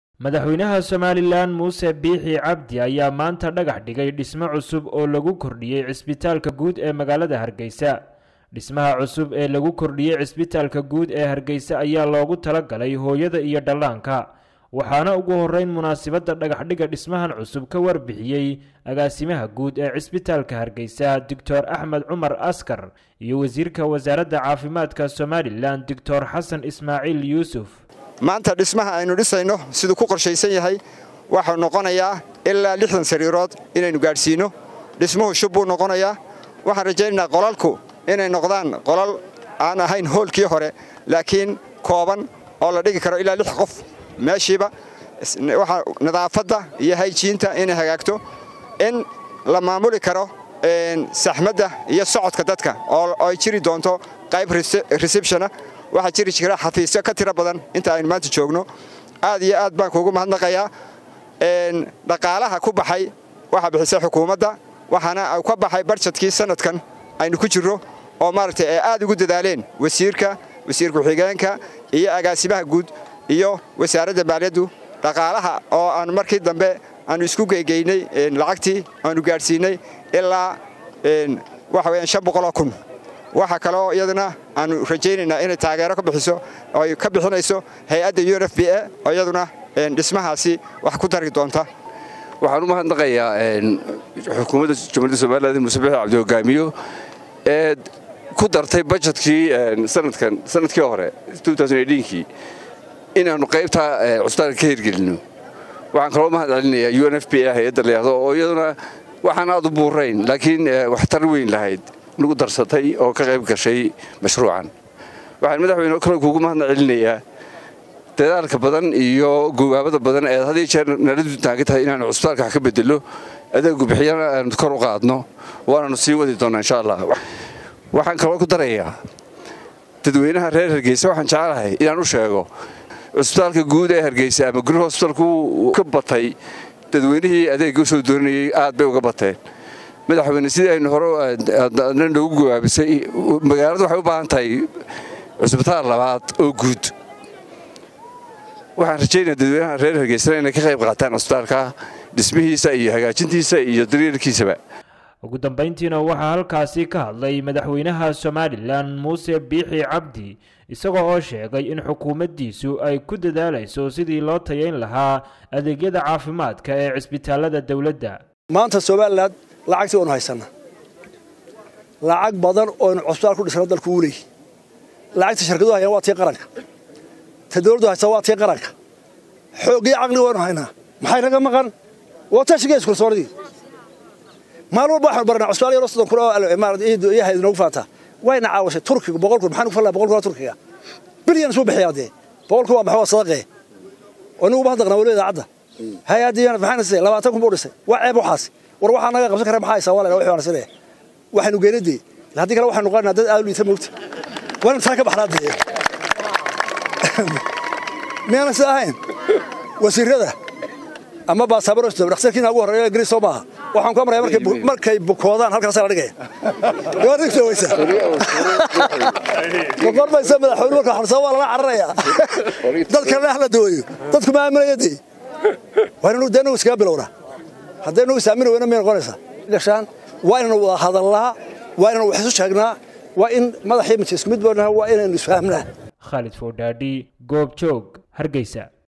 Madaxweynaha Somaliland ayaa dhagax-dhigay waadh cusub oo loogu talo-galay hooyada iyo dhallaanka
Madaxweynaha Somaliland ayaa dhagax-dhigay waadh cusub oo loogu talo-galay hooyada iyo dhallaanka kaasi oo qayb ka ah cisbitaalka guud ee Hargeysa